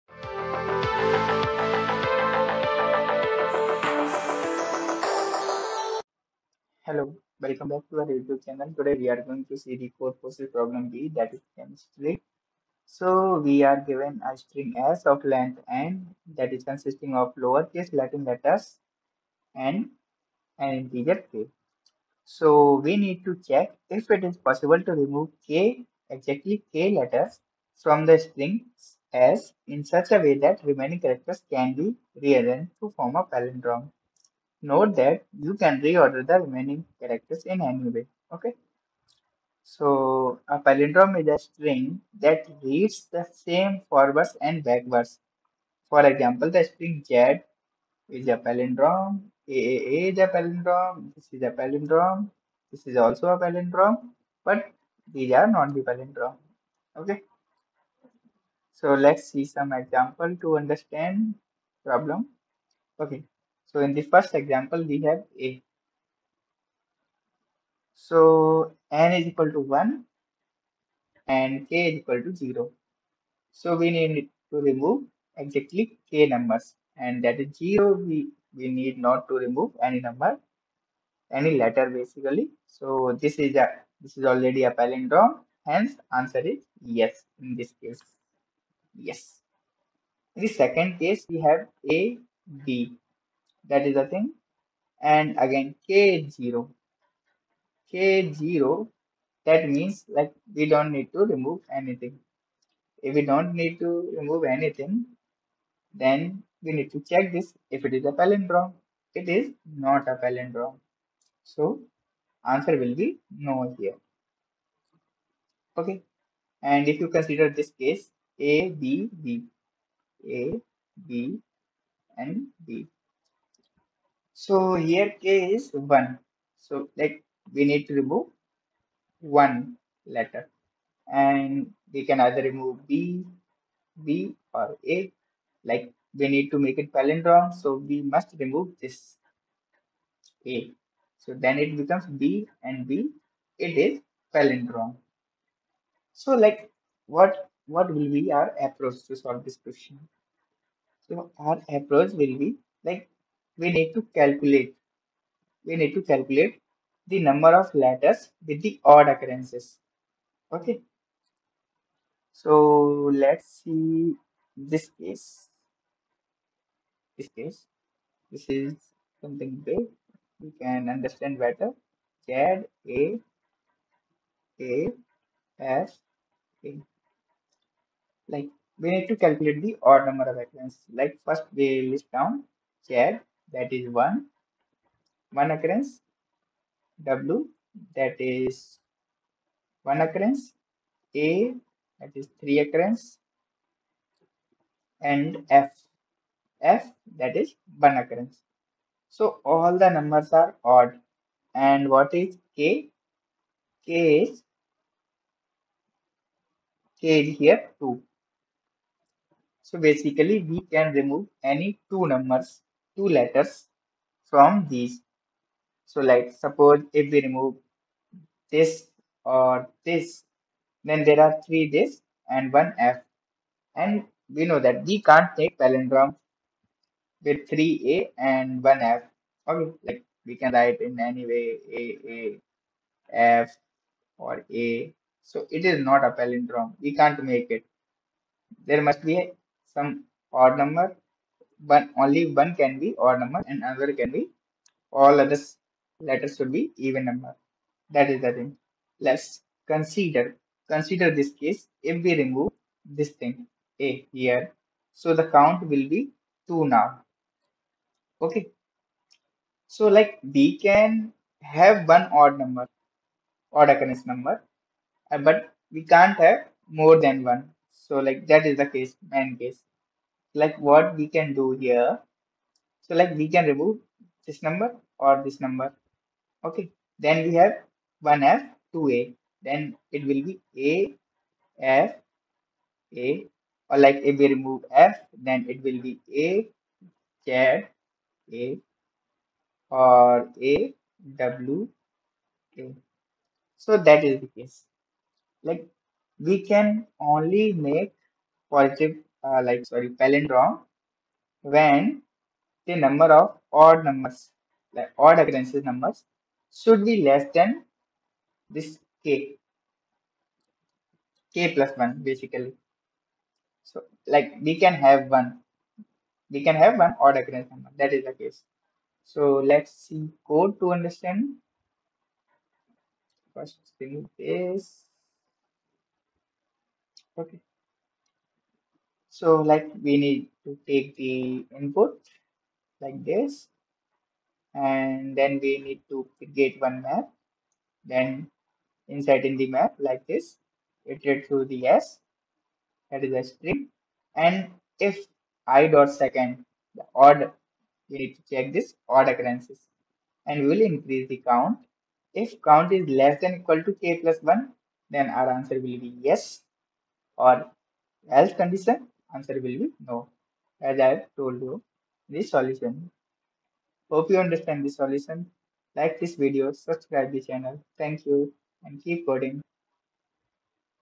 B. Chemistry | Codeforces Round 903 (Div. 3) | Full solution
Two Vessels | Codeforces CodeTON Round #903 (Div. 3 Div. 2)